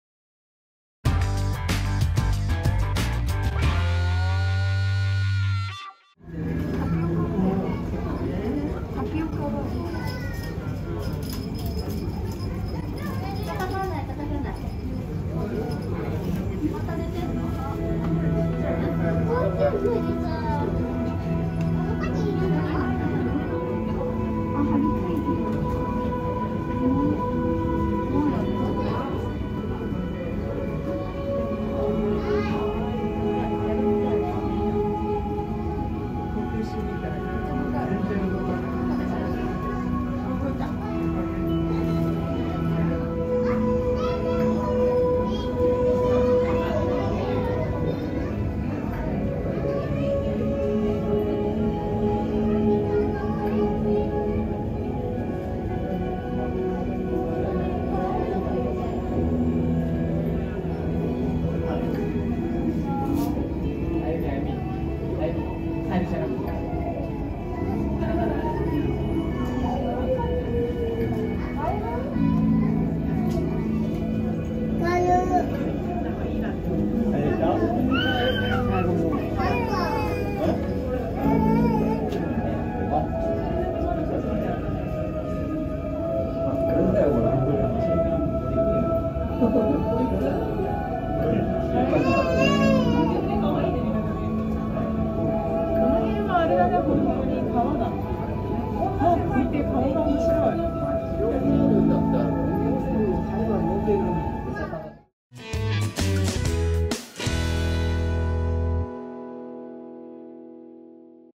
ウーパールーパー（メキシコ サンショウウオ）【サンシャイン水族館】かわいい顔！メキシコサラマンダー アホロートル 2022.7.27【水族館・動物園の生き物動画】東京 Tokyo Japan
サンシャイン水族館（東京・池袋）
・メキシコ サンショウウオ（ウーパールーパー）

こちらを見つめて寄って来ました.

エサがもらえると思ったかな？

『ウーパールーパー』という名前は、日本で独自に付けられた商標名で、海外では通じないそうです.

ピンク色のウーパールーパーはリューシスティックという色素の少ない個体を品種改良したもので、本来の色はマーブルのような黒っぽい体色.

よく見かけるかわいい姿は幼体（オタマジャクシ）で、本来ならサンショウウオのような姿になるはずが、幼体のまま成熟する生態の持ち主.
（幼形成熟（ネオテニー））

まれに成体に変化します.

【どうぶつフレンズ】
動物のオリジナルデザイングッズがAmazonから販売されています.